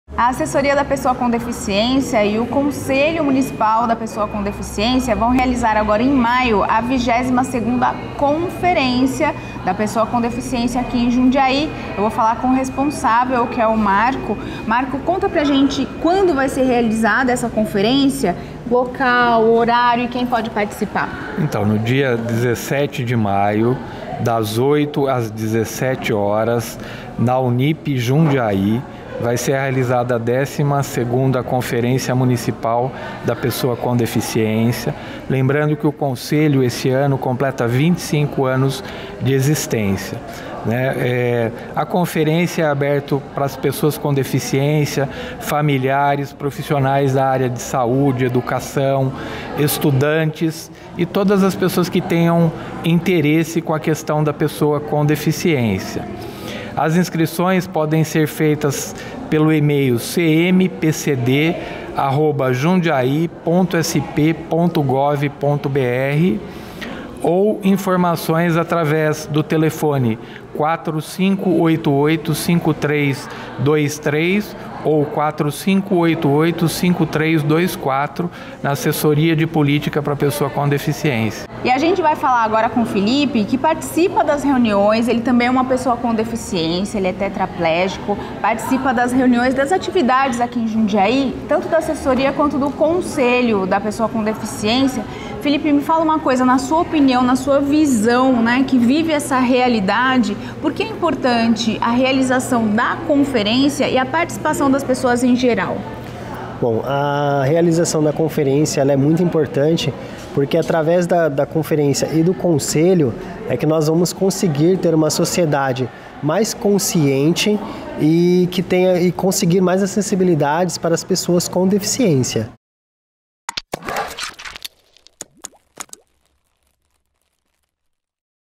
Conferência Municipal da Pessoa com Deficiência
Vem aí 22ª Conferência Municipal da Pessoa com Deficiência em Jundiaí. Aperte o play e saiba como participar!